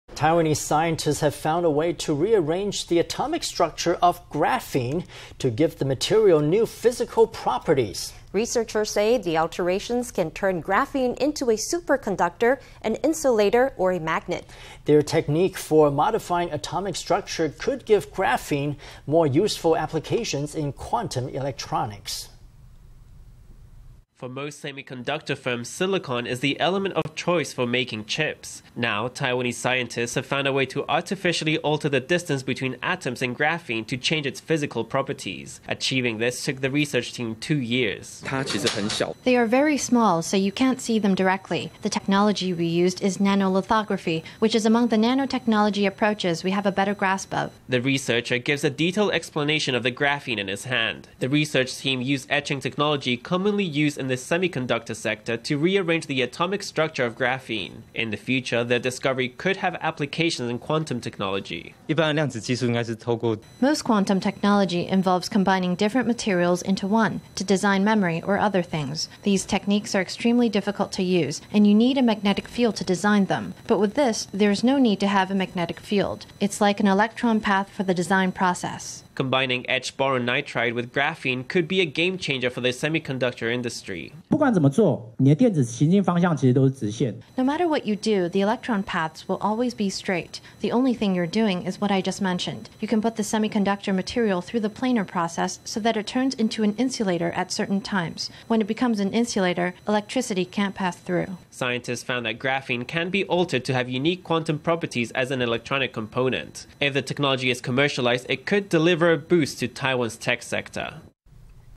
Taiwan scientists develop new graphene properties through atomic alteration
Taiwanese scientists have found a way to rearrange the atomic structure of graphene to give the material new physical properties. Researchers say the alterations can turn graphene into a superconductor, an insulator, or a magnet. Their technique for modifying atomic structure could give graphene more useful applications in quantum electronics.

For most semiconductor firms, silicon is the element of choice for making chips. Now, Taiwanese scientists have found a way to artificially alter the distance between atoms in graphene to change its physical properties. Achieving this took the research team two years.

Hsieh Yu-chiang
Researcher
They are very small, so you can’t see them directly. The technology we used is nanolithography, which is among the nanotechnology approaches we have a better grasp of.

The researcher gives a detailed explanation of the graphene in his hand. The research team used etching technology commonly used in the semiconductor sector to rearrange the atomic structure of graphene. In the future, their discovery could have applications in quantum technology.

Chang Ching-hao
National Cheng Kung University
Most quantum technology involves combining different materials into one, to design memory or other things. These techniques are extremely difficult to use, and you need a magnetic field to design them. But with this, there’s no need to have a magnetic field. It’s like an electron path for the design process.

Combining etched boron nitride with graphene could be a game changer for the semiconductor industry.

Chen Tse-ming
No matter what you do, the electron paths will always be straight. The only thing you’re doing is what I just mentioned. You can put the semiconductor material through the planar process so that it turns into an insulator at certain times. When it becomes an insulator, electricity can’t pass through.

Scientists found that graphene can be altered to have unique quantum properties as an electronic component. If the technology is commercialized, it could deliver a boost to Taiwan’s tech sector.